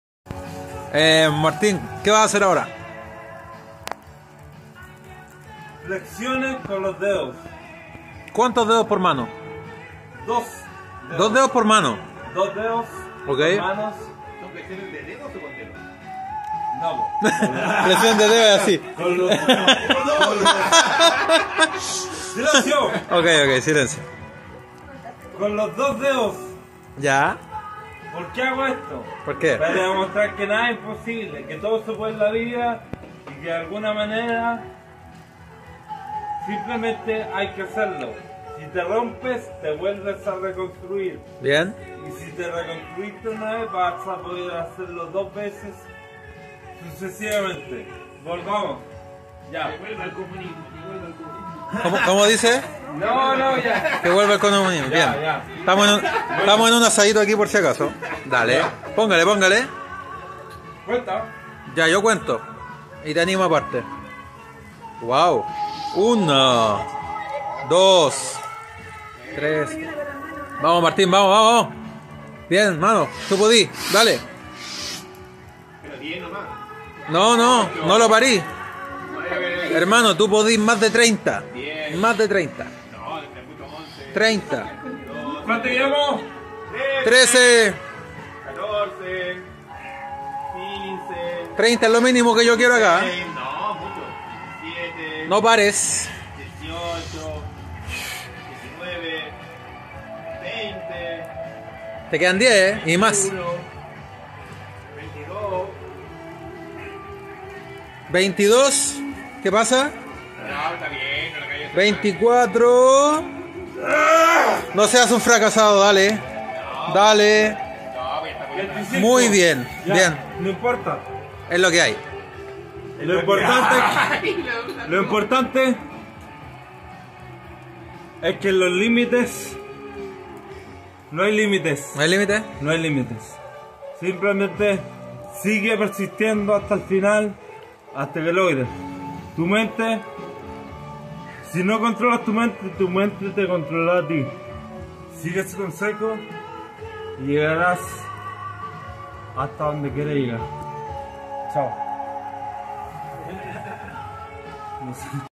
Ray Saenger, cantante chileno: 25 flexiones con 2 dedos por mano ¡No hay límites!
Hola, me llamo Ray Saenger y soy cantante.
Soy chileno, practico calistenia y soy aficionado al ajedrez.